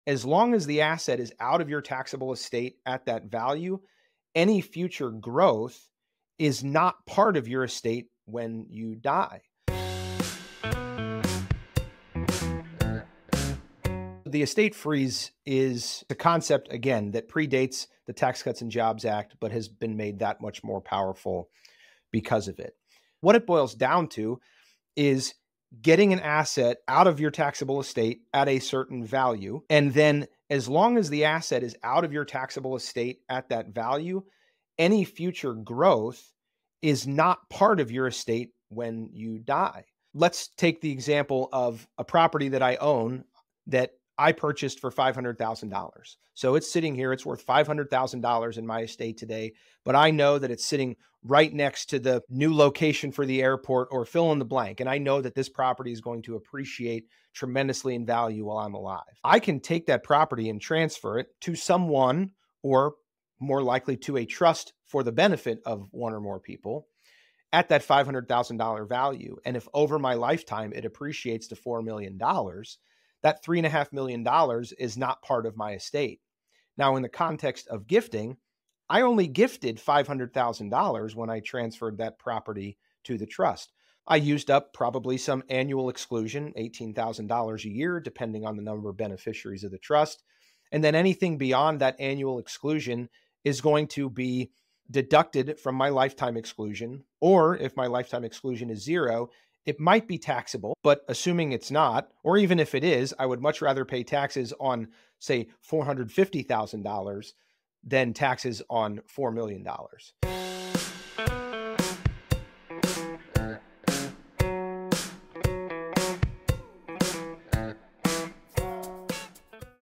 Estate Tax Freeze
How you can remove appreciation of assets from your estate, saving your 40% in taxes.